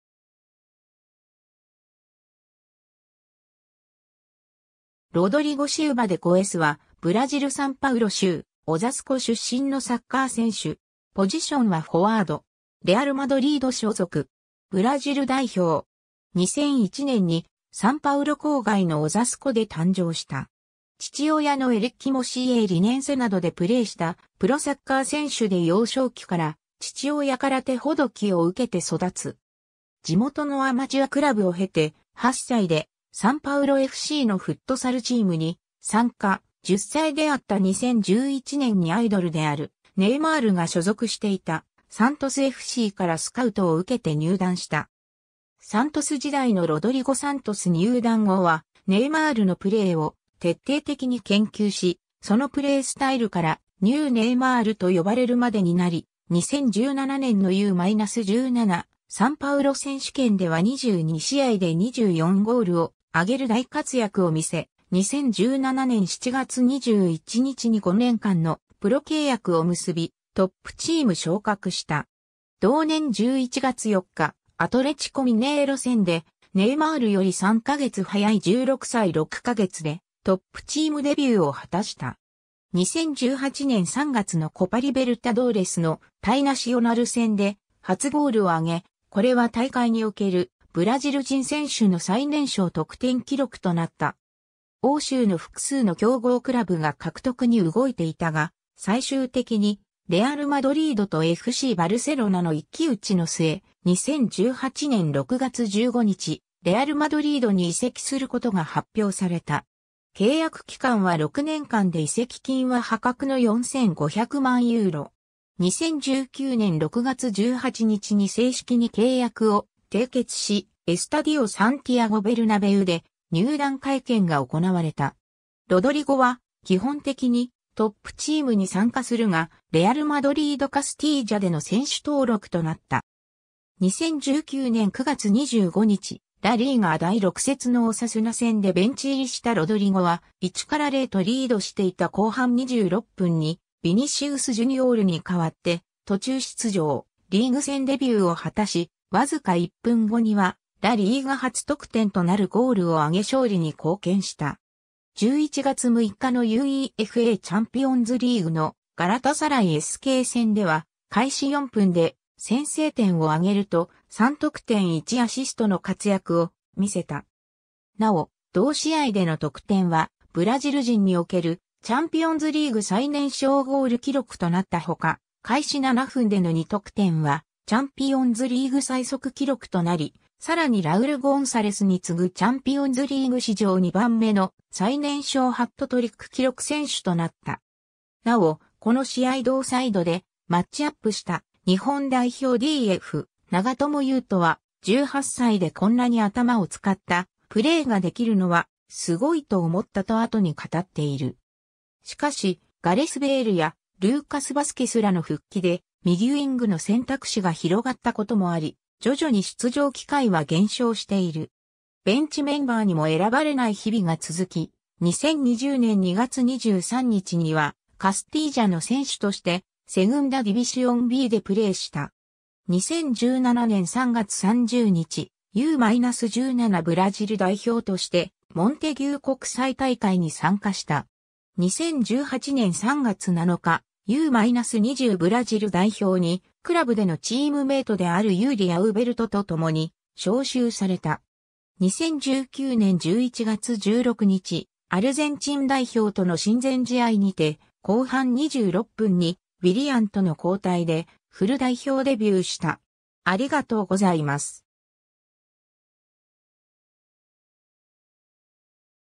ロドリゴ・シウバ・デ・ゴエス
ロドリゴ・シウバ・デ・ゴエス（Rodrygo Silva de Goes, 2001年1月9日 - ）は、ブラジル・サンパウロ州オザスコ出身のサッカー選手。
レアル・マドリード所属。
 2001年にサンパウロ郊外のオザスコで誕生した。
 父親のエリッキもCAリネンセなどでプレーしたプロサッカー選手（2018年に現役引退）で幼少期から父親から手ほどきを受けて育つ。
地元のアマチュアクラブを経て、8歳でサンパウロFCのフットサルチームに参加、10歳であった2011年にアイドルであるネイマールが所属していたサントスFCからスカウトを受けて入団した。
 サントス時代のロドリゴ（2018年） サントス入団後はネイマールのプレーを徹底的に研究し、そのプレースタイルから”NEWネイマール”と呼ばれるまでになり、2017年のU-17サンパウロ選手権では22試合で24ゴールを挙げる大活躍を見せ、2017年7月21日に5年間のプロ契約を結び、トップチーム昇格した。
同年11月4日、アトレチコ・ミネイロ戦でネイマールより3ヵ月早い16歳6ヵ月でトップチームデビューを果たした。
2018年3月のコパ・リベルタドーレスの対ナシオナル戦で初ゴールを挙げ、これは大会におけるブラジル人選手の最年少得点記録となった（17歳2ヶ月）。
 欧州の複数の強豪クラブが獲得に動いていたが、最終的にレアル・マドリードとFCバルセロナの一騎打ちの末、2018年6月15日、レアル・マドリードに移籍することが発表された。
契約期間は6年間で移籍金は破格の4500万ユーロ（当時のレートで約58億円）。
2019年6月18日に正式に契約を締結し、エスタディオ・サンティアゴ・ベルナベウで入団会見が行われた。
ロドリゴは基本的にトップチームに参加するがレアル・マドリード・カスティージャでの選手登録となった。
 2019年9月25日、ラ・リーガ第6節のオサスナ戦でベンチ入りしたロドリゴは、1-0とリードしていた後半26分にヴィニシウス・ジュニオールに代わって途中出場、リーグ戦デビューを果たし、僅か1分後にはラ・リーガ初得点となるゴールを挙げ勝利に貢献した。
11月6日のUEFAチャンピオンズリーグのガラタサライSK戦では開始4分で先制点を挙げると、3得点1アシストの活躍を見せた。
なお、同試合での得点はブラジル人におけるチャンピオンズリーグ最年少ゴール記録となったほか、開始7分での2得点はチャンピオンズリーグ最速記録となり、更にラウル・ゴンサレスに次ぐチャンピオンズリーグ史上2番目の最年少ハットトリック記録選手となった。
尚、この試合同サイドでマッチアップした、日本代表DF長友佑都は「18歳でこんなに頭を使ったプレーができるのは凄いと思った」と後に語っている。
 しかし、ガレス・ベイルやルーカス・バスケスらの復帰で右ウイングの選択肢が広がったこともあり徐々に出場機会は減少している。
ベンチメンバーにも選ばれない日々が続き、2020年2月23日にはカスティージャの選手としてセグンダ・ディビシオンBでプレーした。
 2017年3月30日、U-17 ブラジル代表としてモンテギュー国際大会に参加した。
2018年3月7日、U-20ブラジル代表にクラブでのチームメイトであるユーリ・アウベルトと共に招集された。
 2019年11月16日、アルゼンチン代表との親善試合にて、後半26分にウィリアンとの交代でフル代表デビューした。